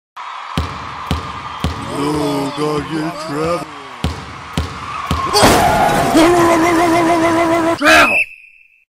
Travel!
The definition of Travel and how it works, in this case, has been changed. This is the new penalty for traveling in basketball.

Also, my first animation ever completed. ;)